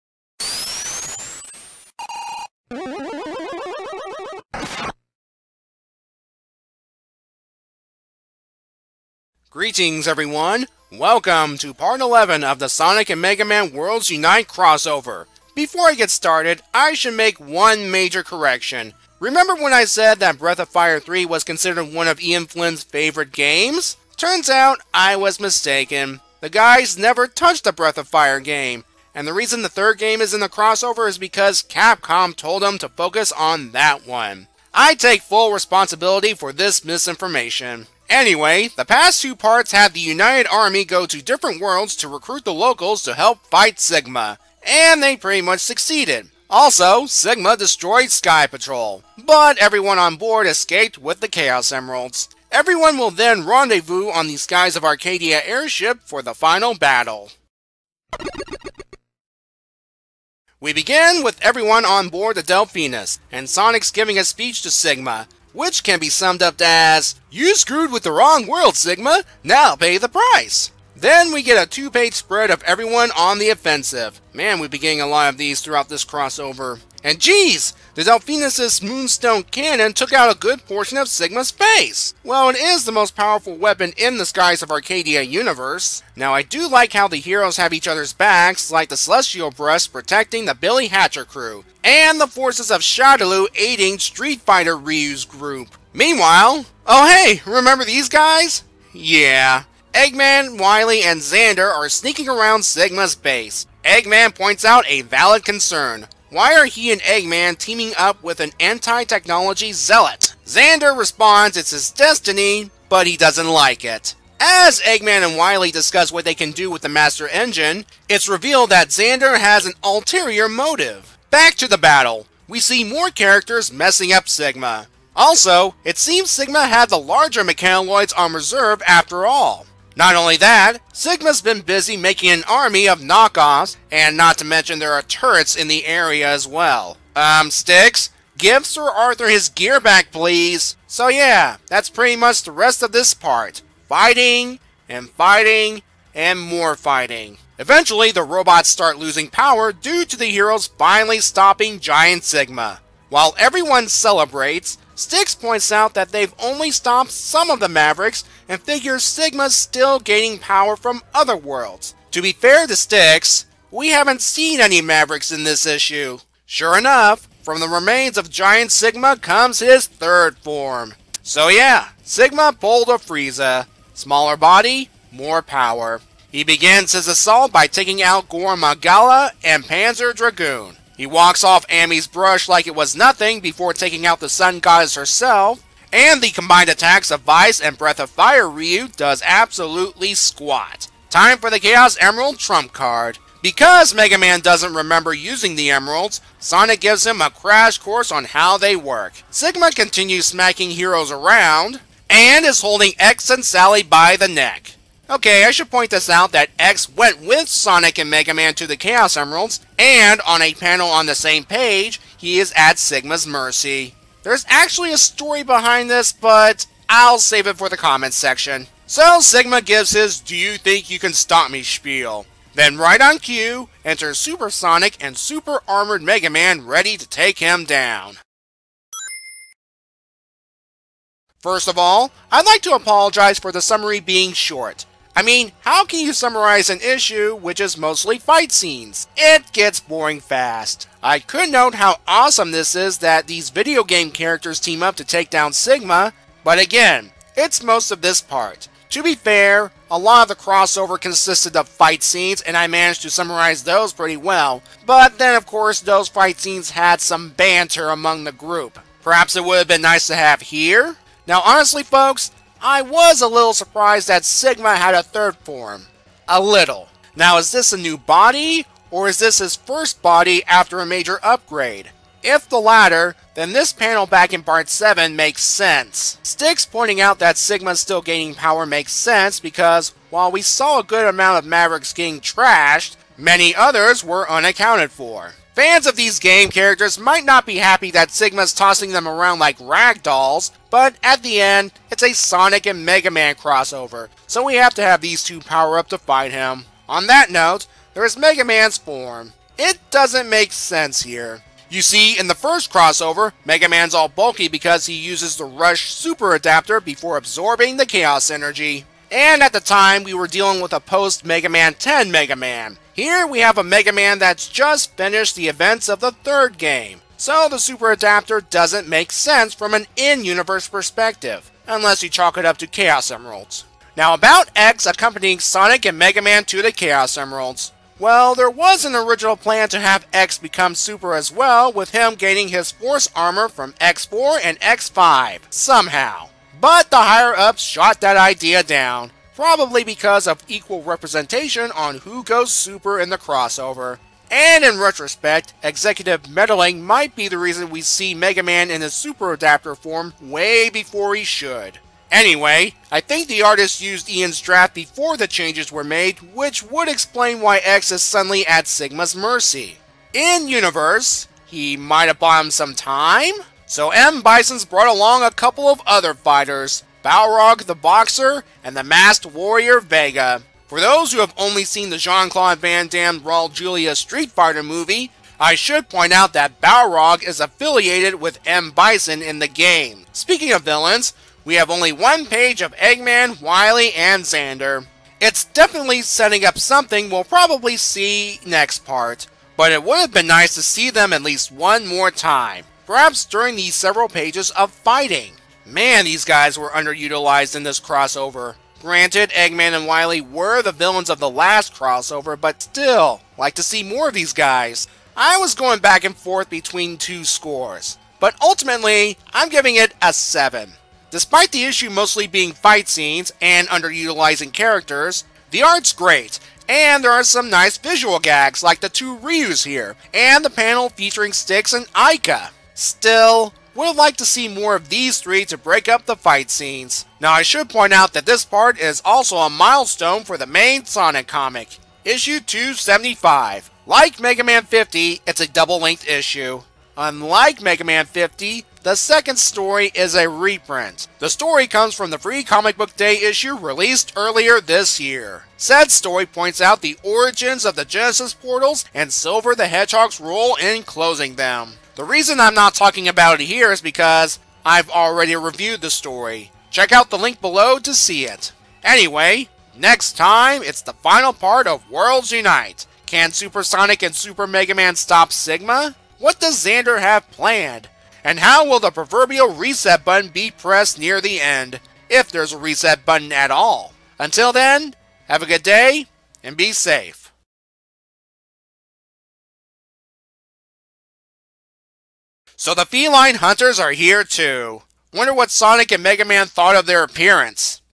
Sonic & Mega Man 2: Worlds Unite (Part 11) - A Comic Review by Megabeatman
In this penultimate part,  the final battle against Sigma begins. But Sigma won't go down so easily.